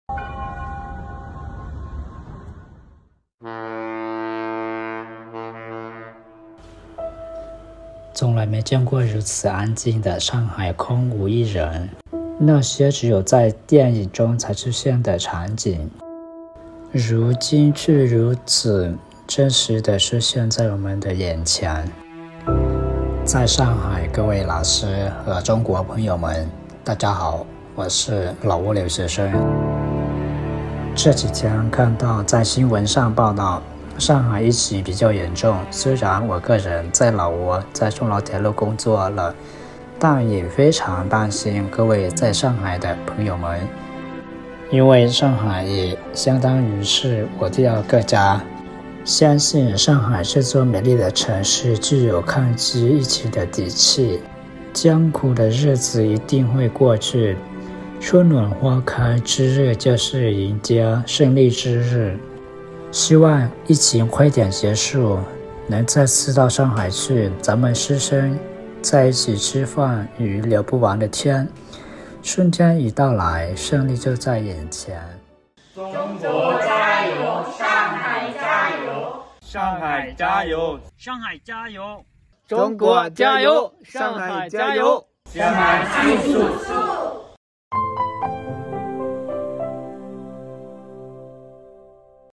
老挝留学生寄来给“第二家乡”上海的加油祝福
疫情让上海这座城市静了下来，正在中老铁路工作和实习的上海应用技术大学铁道工程专业的老挝留学生，从新闻报道上得知上海防控新冠疫情进入最关键阶段，一刻也无法平静，他们深深牵挂着上海这个第二故乡，牵挂着这里的老师、同学和中国朋友们。

他们第一时间编辑视频祝福上海，感谢上海对他们学习和生活上的帮助，用老挝家乡话为上海加油，带领身边的老挝朋友们一起为上海打气。他们相信上海这座美丽的城市具有抗击疫情的底气，坚信艰难的日子一定会过去，春天已到来，胜利就在眼前。

“中国加油、上海加油、上海susu！”疫情让我们相隔两端，关怀却从不会缺席，心心相印，共克时艰，这正是中老友谊最好的诠释。（吕客 视频来源：上海应用技术大学）